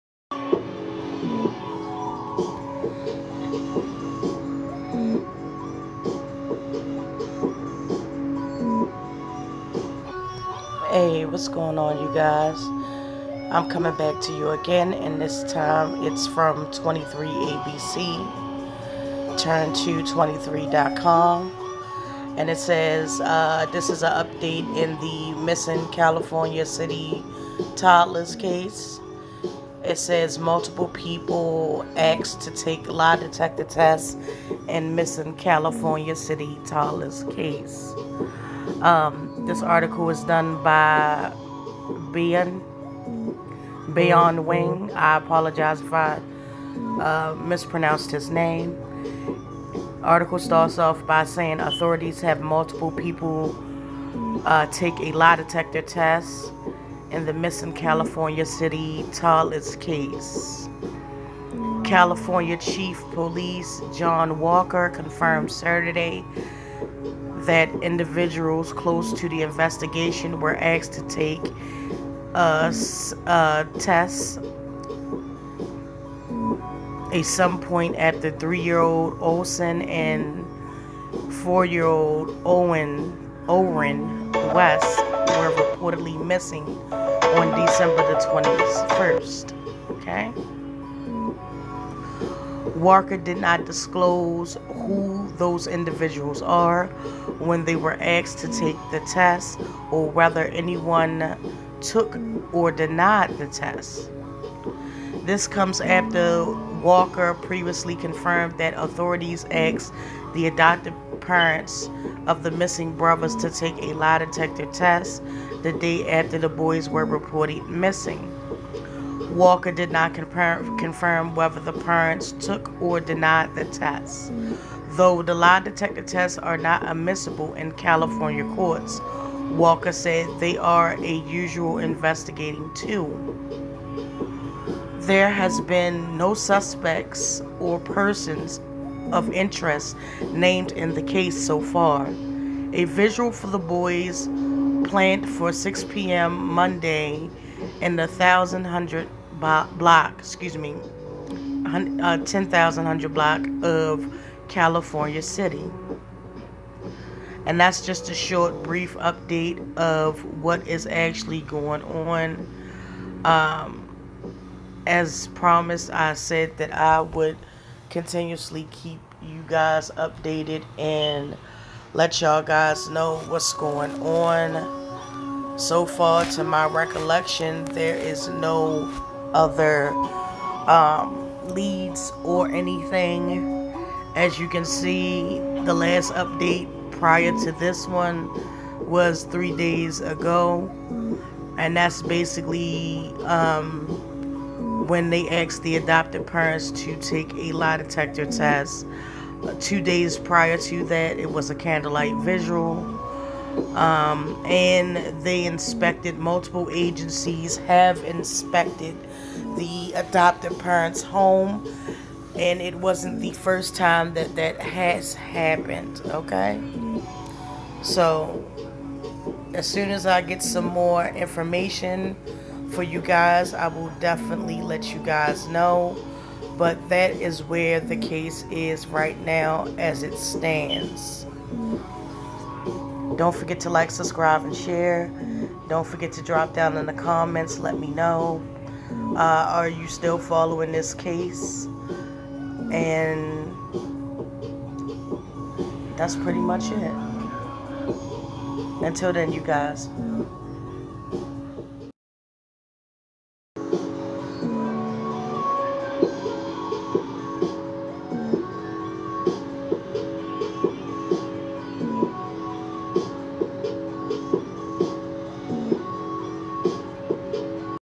Multiple people asked to take lie detector tests in missing California City toddlers case.
OrsonWest Age 3
OrrinWest Age 4
💎They were reported missing on Dec. 21.